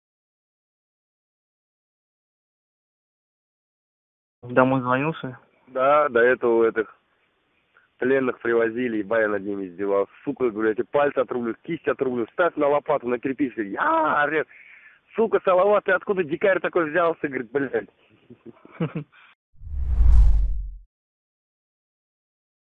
россияне хвастают, как пытают украинских военнопленных. Война на Украине с россией в 2022 году.
росіяни вихваляються, як катують українських військовополонених. Війна в Україні з росією у 2022 році.

russians boast of torturing Ukrainian prisoners of war. The war in Ukraine with russia in 2022.